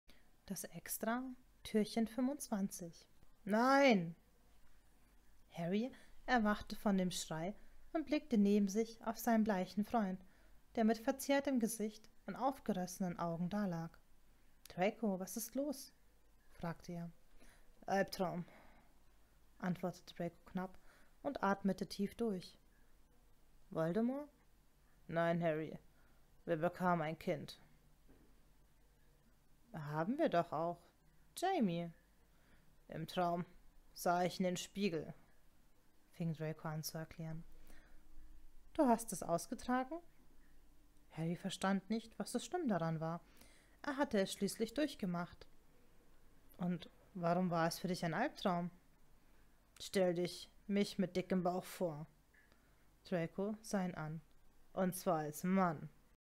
Drarry Adventskalender 2020 ~ Harry, Potter, Draco, Malfoy - Ja was den nun? - Türchen 25
Adventskalender Special 2020
Drarry Fanfiktion
Drachen-Fan - Harry, Potter, Malfoy, Draco - Ja was den nun?

Kurzbeschreibung:

Irgendwie dreht sich alles um Harry und Draco, oder auch Potter und Malfoy.